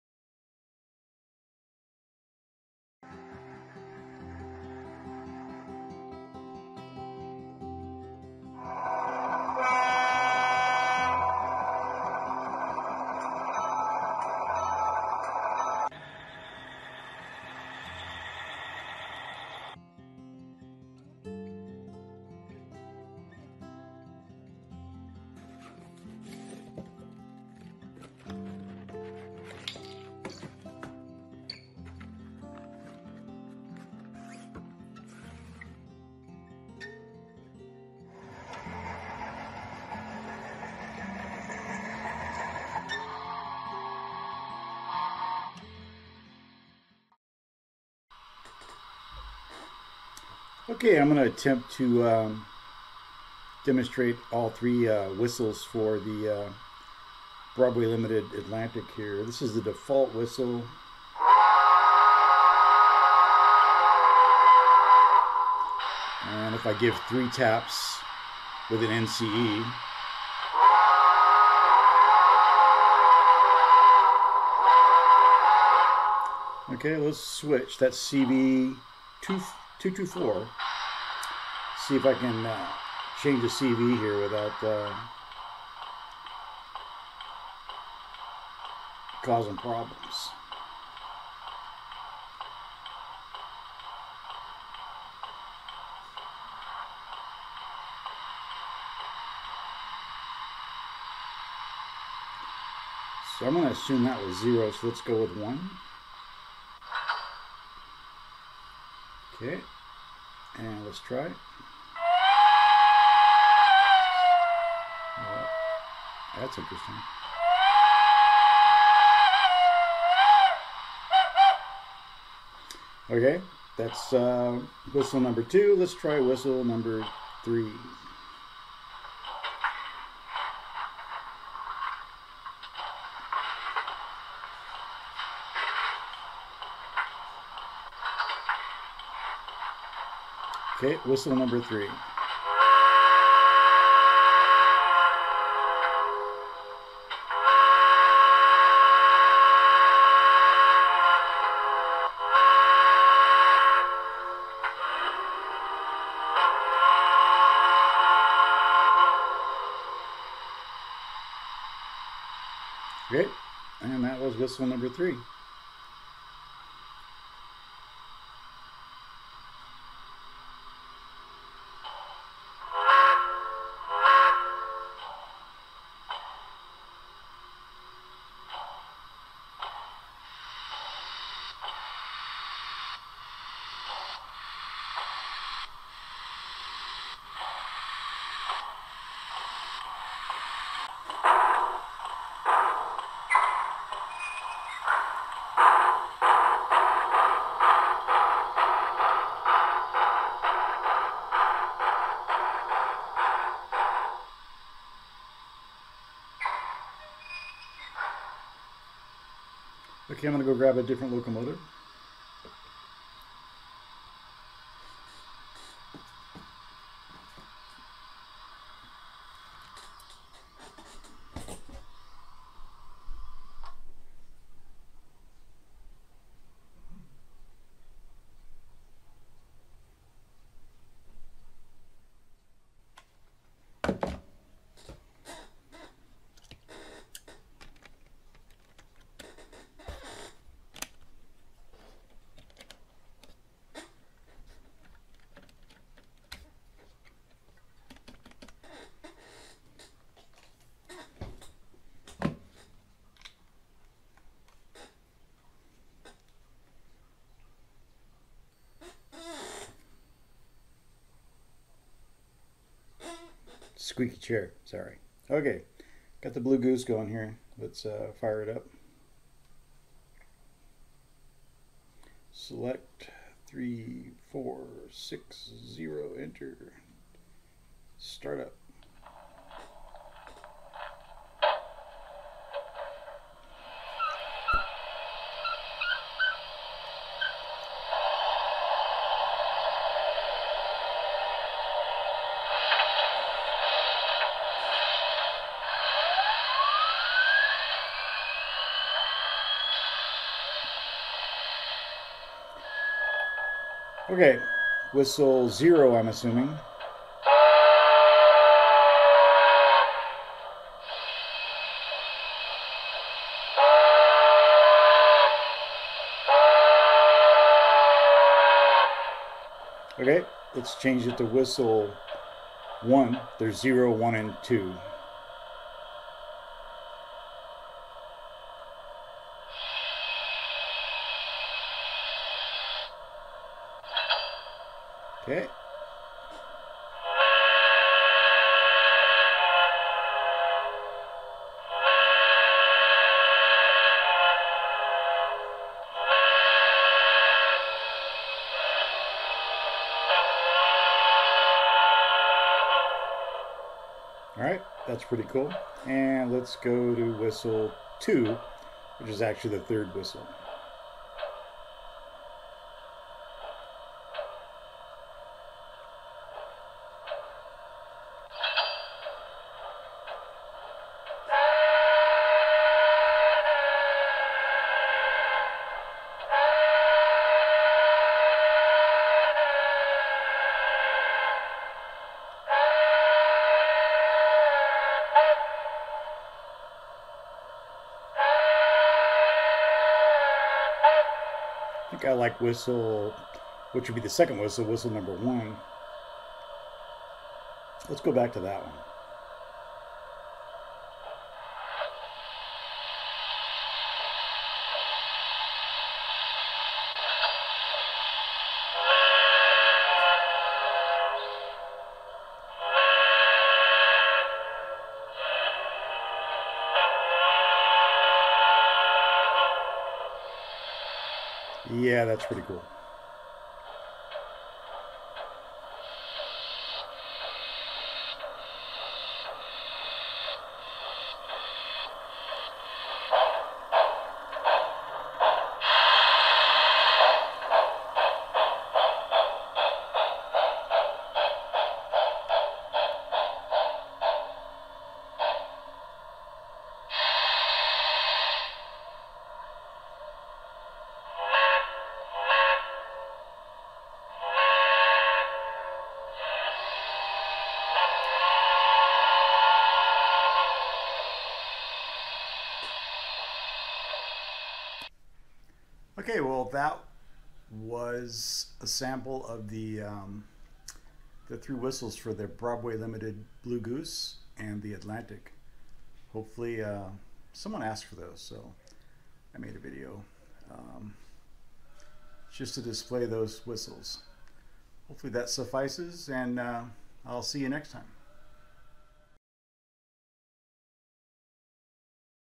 Broadway Limited Blue Goose and Atlantic whistle options
Here Im playing all three whistle options for these two locomotives.